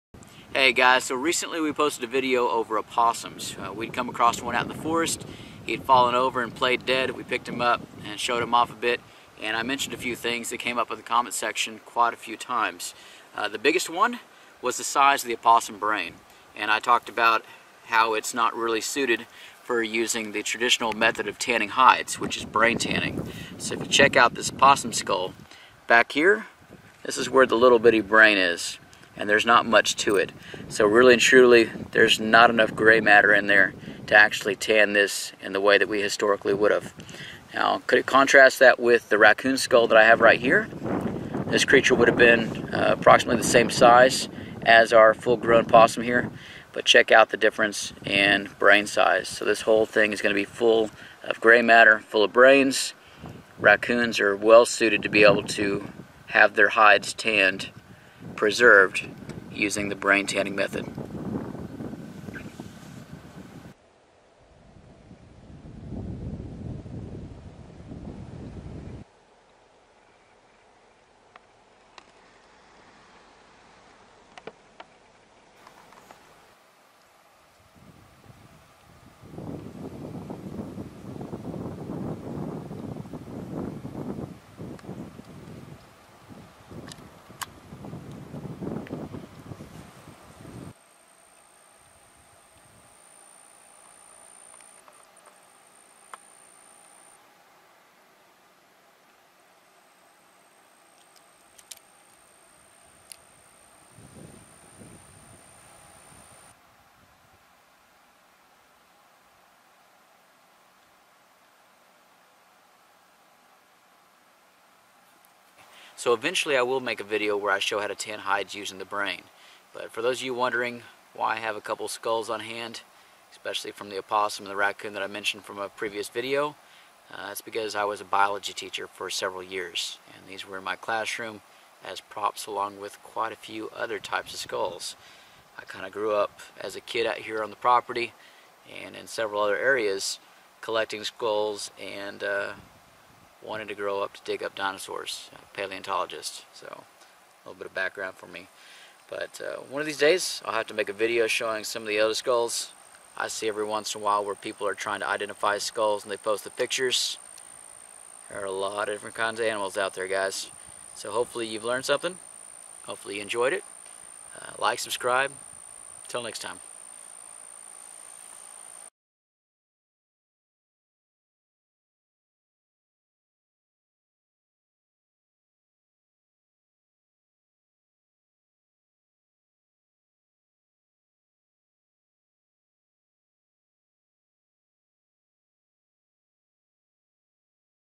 Opossum vs Raccoon -Brain Size-
Follow-up video in response to questions over the opossum brain size and its suitability for brain tanning.

There were no creatures harmed in the making of this video.  The skulls were part of a biology classroom display.